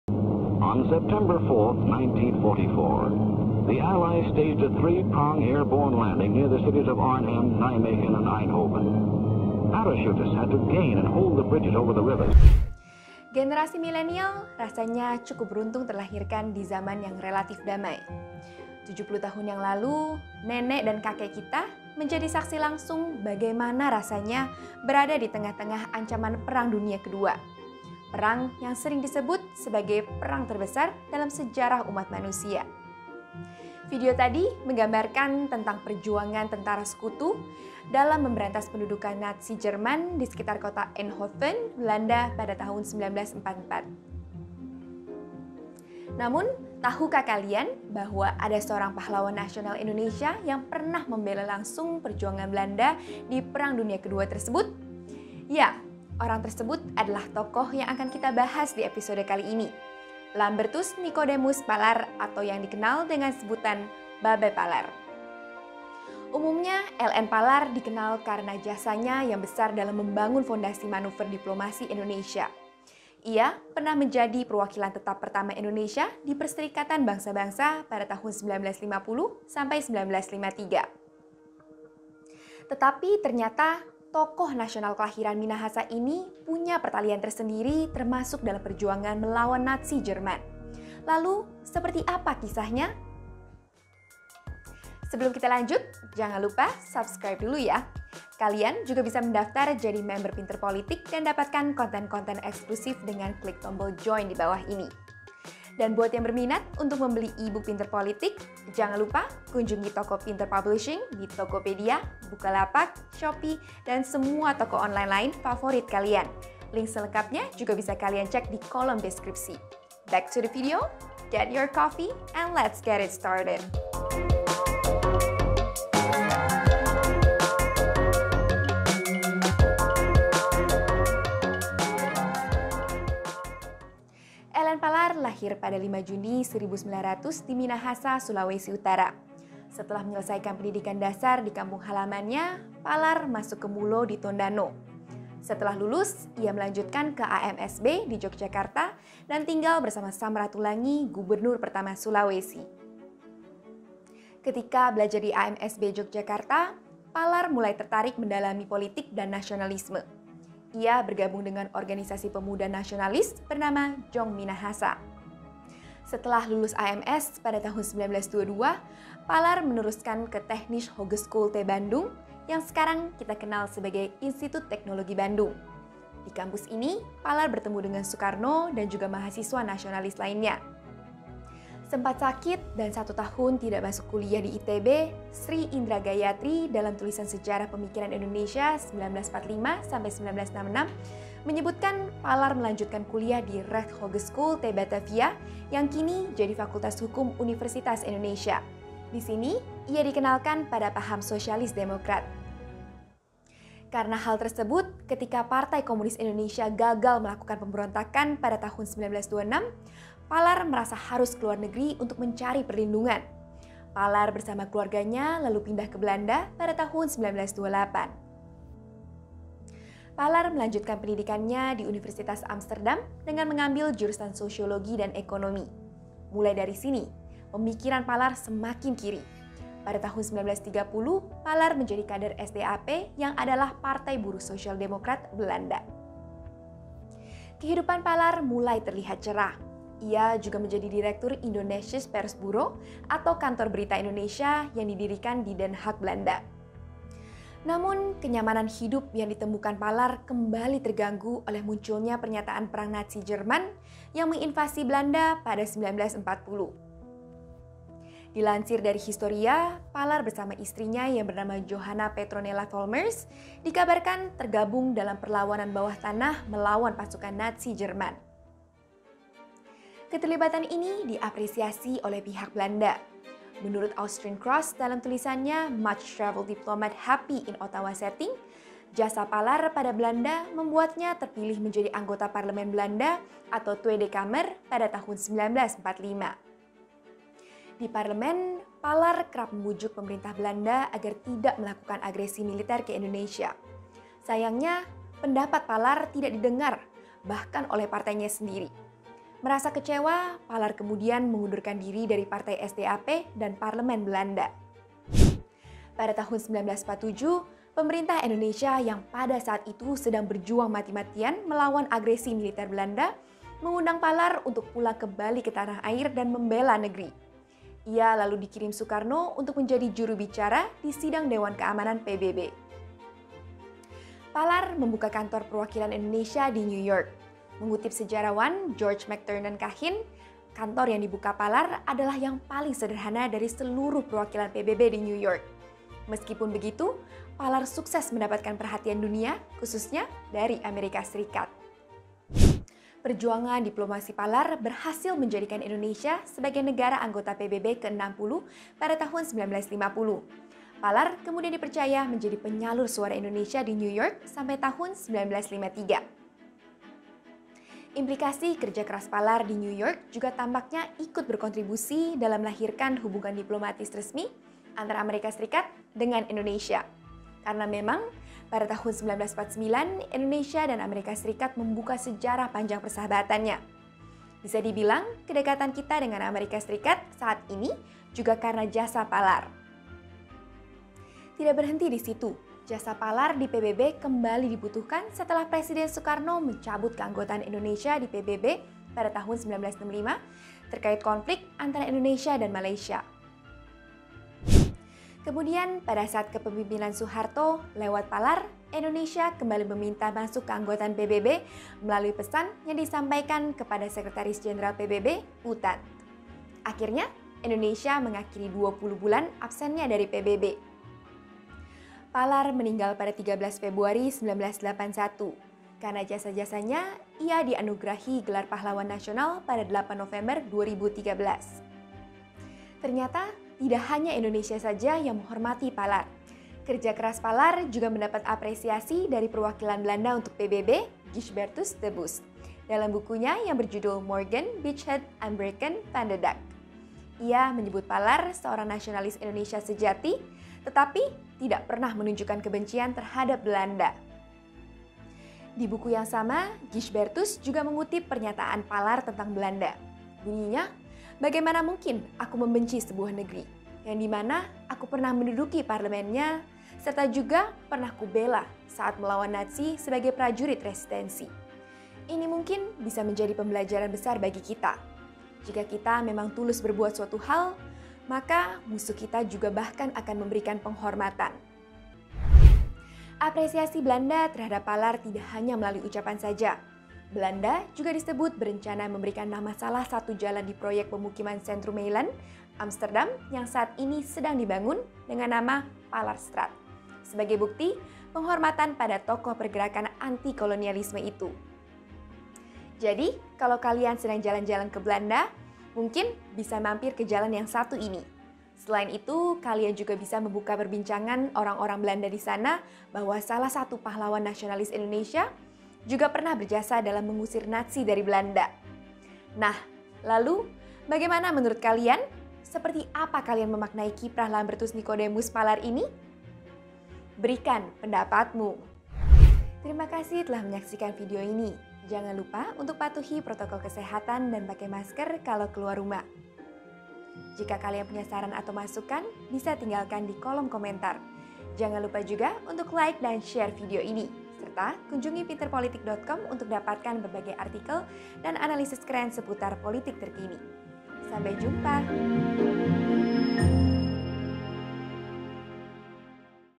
Sejarah LN Palar: Diplomat Nusantara Pemberantas Nazi?
Lambertus Nicodemus Palar atau yang dikenal dengan panggilan Babe Palar, Seorang Pahlawan Nasional Indonesia yang pernah membela langsung perjuangan Belanda di Perang Dunia ke-2. LN Palar dikenal karena jasanya yang besar dalam membangun fondasi manuver diplomasi Indonesia.  Tetapi ternyata, tokoh nasional kelahiran Minahasa ini punya pertalian tersendiri termasuk dalam perjuangan melawan Nazi Jerman. Lalu seperti apa kisahnya?